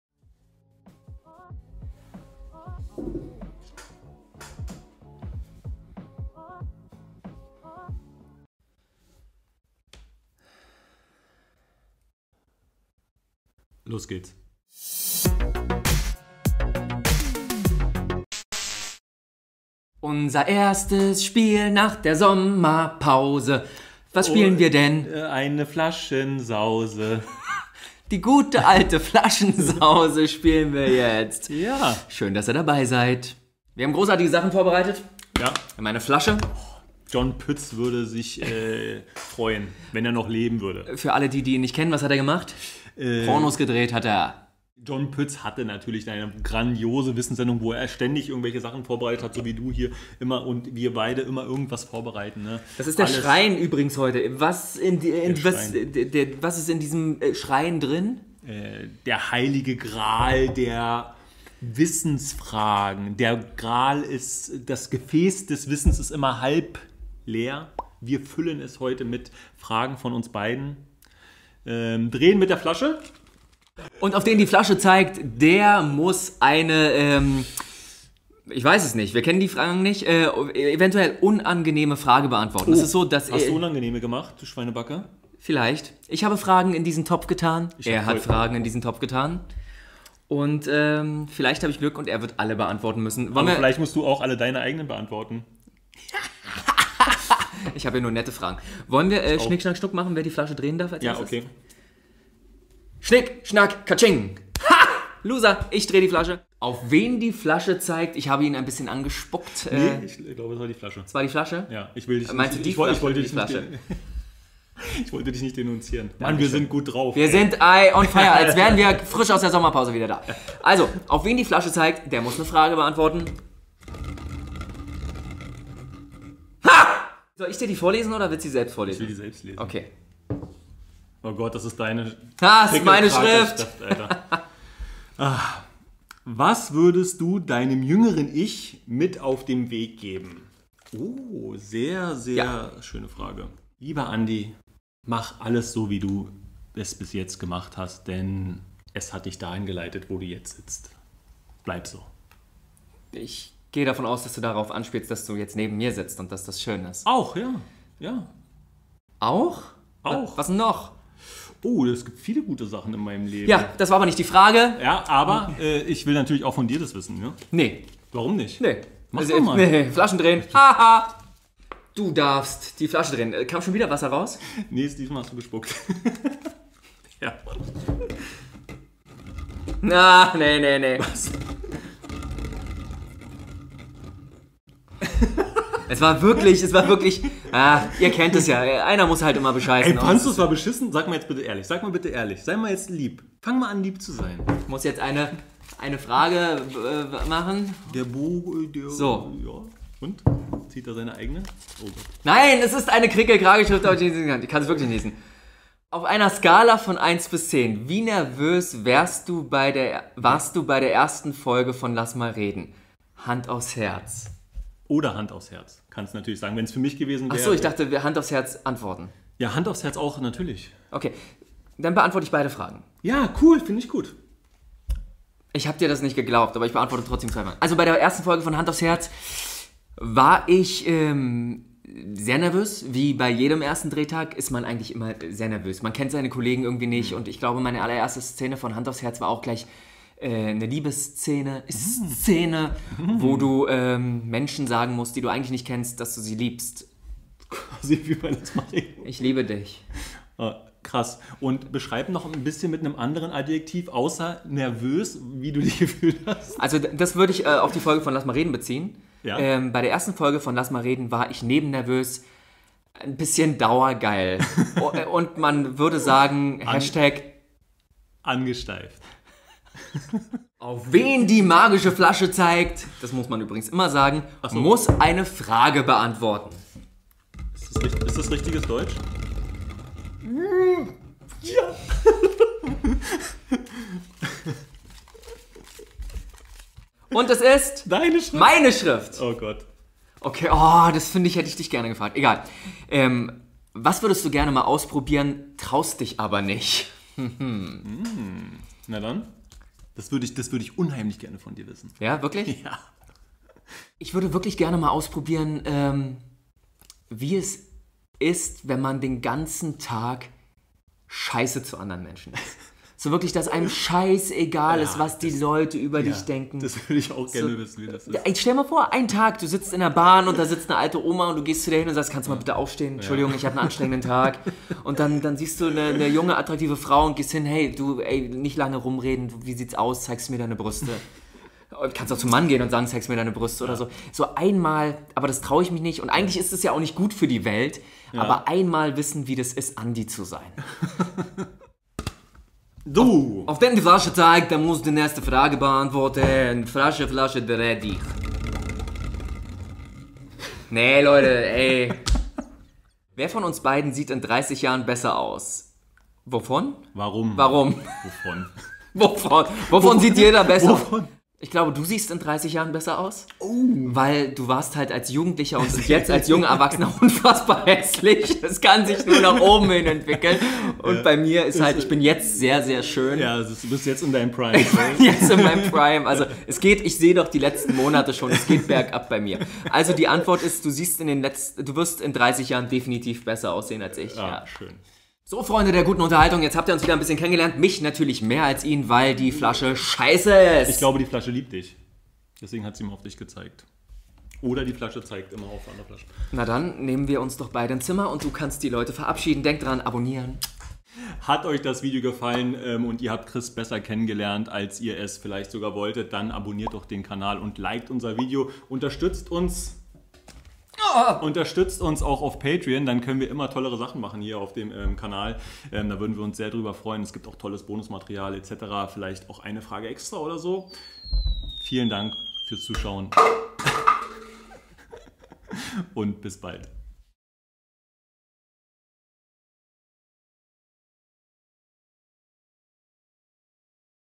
"Hand aufs Herz" Geständnis & time travel selfspeech | Flaschendrehen | The Game | Lass Mal Reden
Hey Leute! Es ist soweit, neue Woche, neues Spiel mit interessnten Geständnissen und vielem mehr. Viel Spaß!